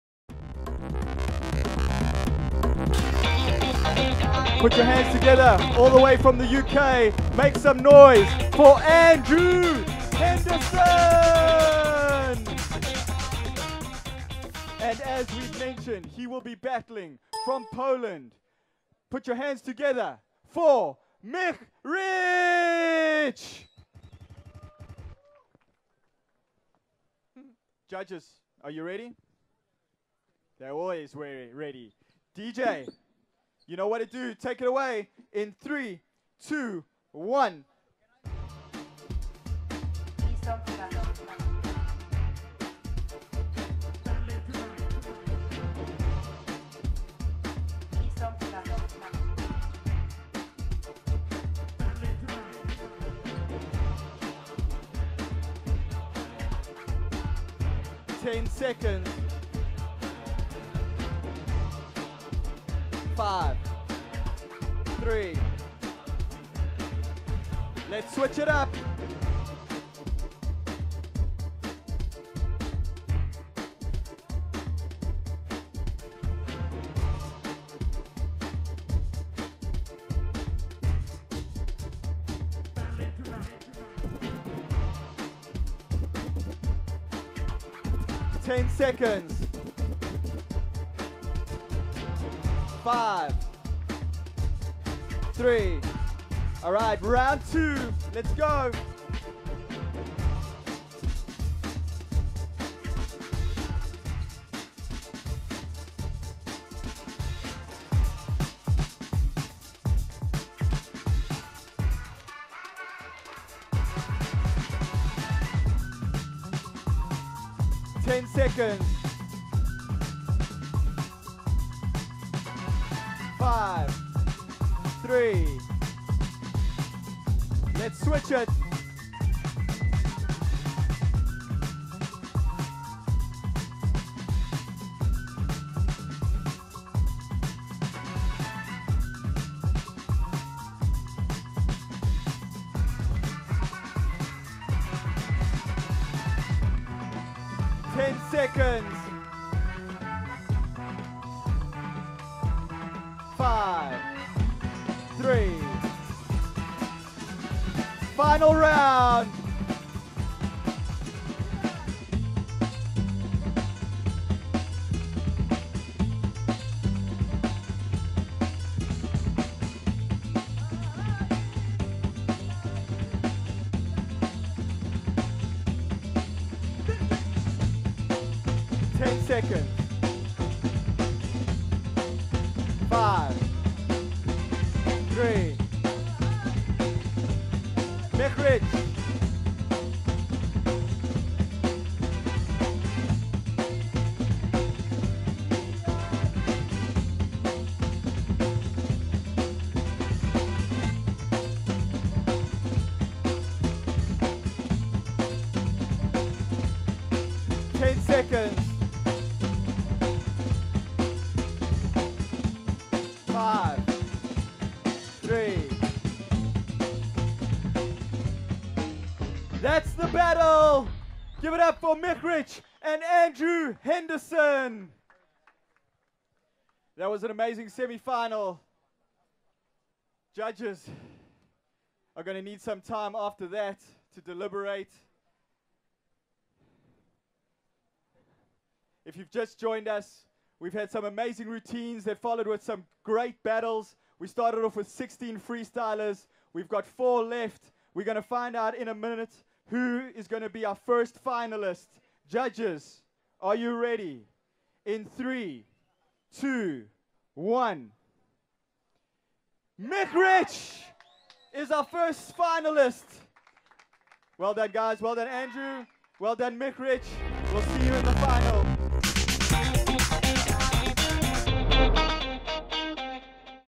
WFFC 2013 Semi Final Battle: Michryc vs Andrew Henderson
Semi final battle during the Head & Shoulders World Freestyle Football Championship - Dubai 2013 (2nd stop during the F3 World Tour 2013)